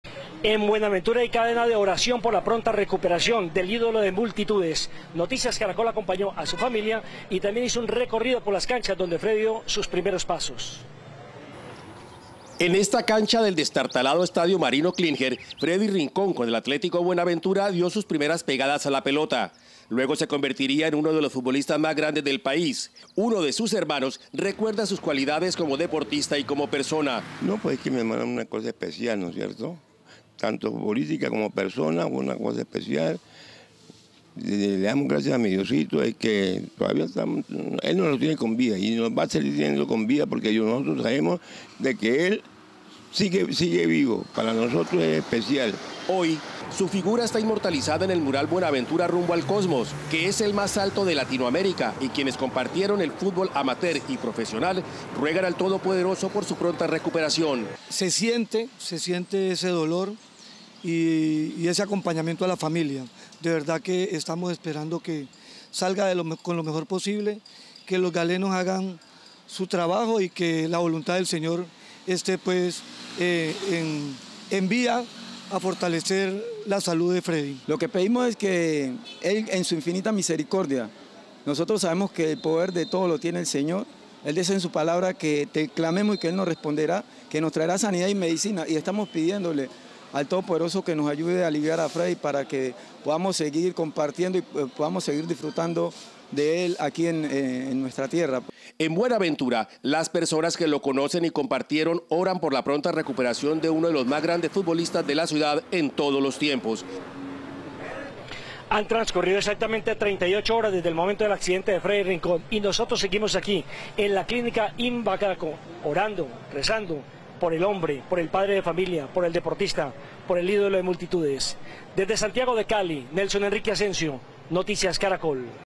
En Buenaventura también están pendientes y mandando energía positiva por la salud de Freddy Rincón
En la tierra natal del otrora jugador de la Selección Colombia, Real Madrid y Corinthians, entre otros, sus familiares y amigos hacen votos por su recuperación.

GolCaracol. Toda la información de las eliminatorias sudamericanas Catar 2022